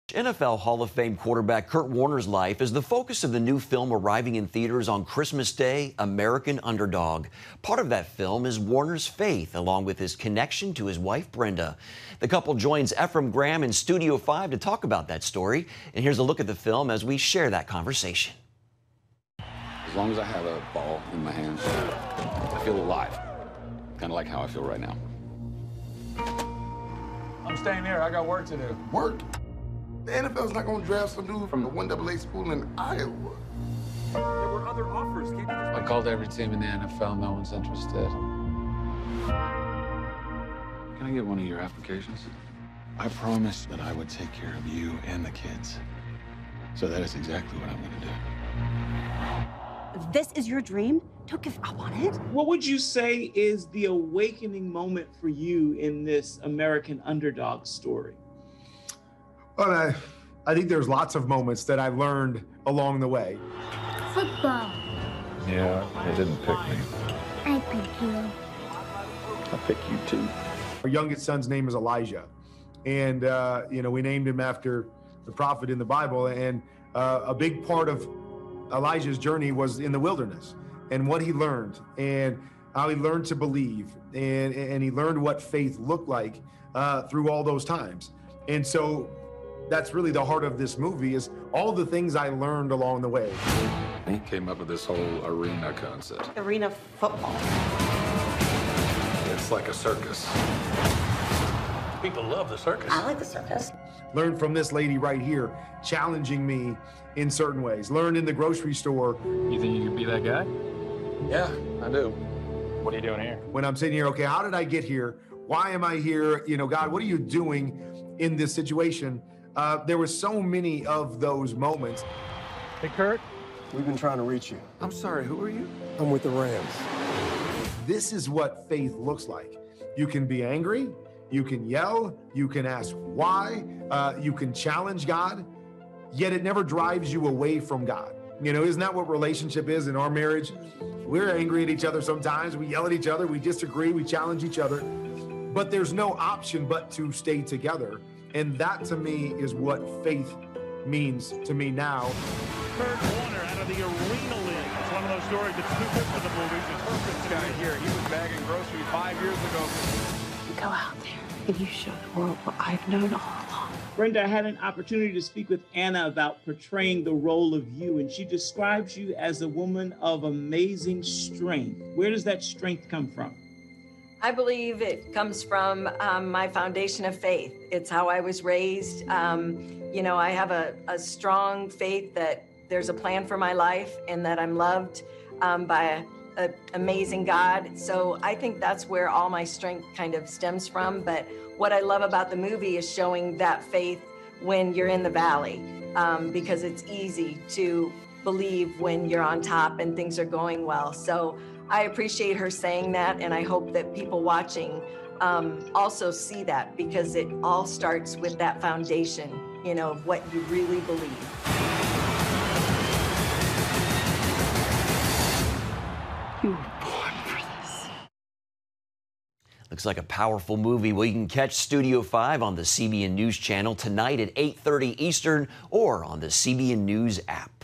'Faith When You're in the Valley': NFL Star Kurt Warner Talks 'American Underdog' and Relying on God
An uplifting film based on the true story of former NFL superstar Kurt Warner and his life before football fame is scheduled for release on Christmas Day.

Warner says the movie, American Underdog: The Kurt Warner Story, is meant to inspire people who dream of becoming better and achieving more. He told CBN News part of his real-life journey was about finding his way through the wilderness before he made it big in the NFL.